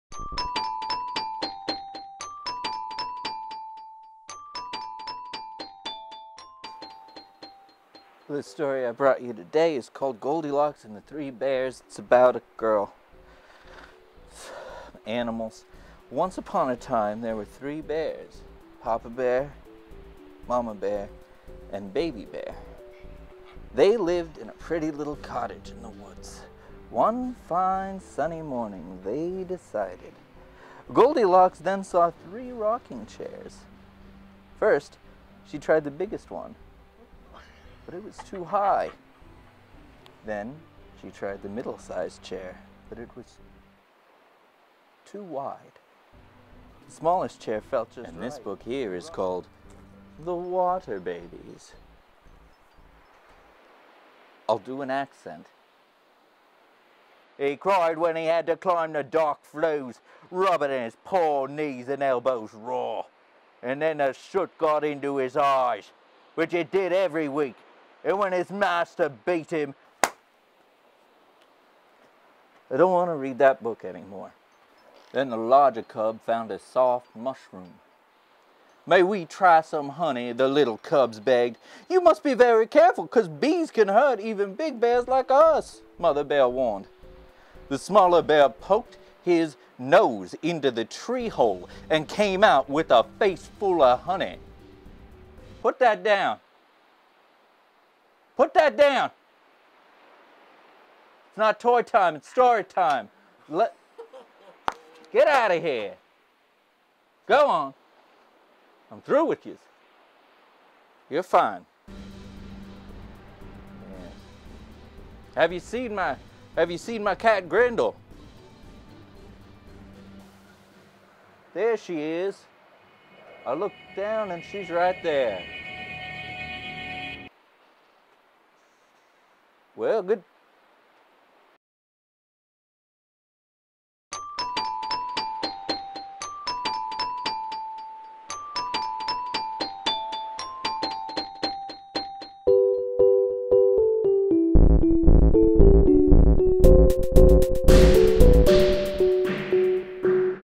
Ians Reads: Starring Grendal
I built a greenscreen set-up in my basement and this is what I done with it
this is